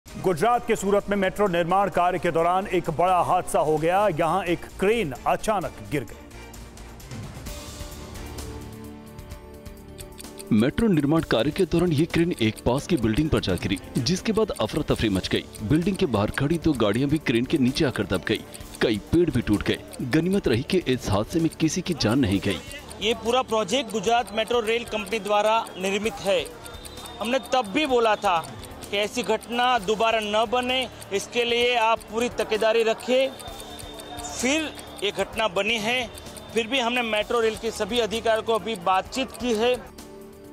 Gujarat News: मेट्रो निर्माण के दौरान हादसा, मकान पर गिरी क्रेन | Gujarat Metro | Gujarat Police
मेट्रो निर्माण कार्य के दौरान ये क्रेन एक पास की बिल्डिंग पर जा गिरी...जिसके बाद अफरा-तफरी मच गई...बिल्डिंग के बाहर खड़ी दो गाड़ियां भी क्रेन के नीचे आकर दब गईं...कई पेड़ भी टूट गए...गनीमत रही कि इस हादसे में किसी की जान नहीं गई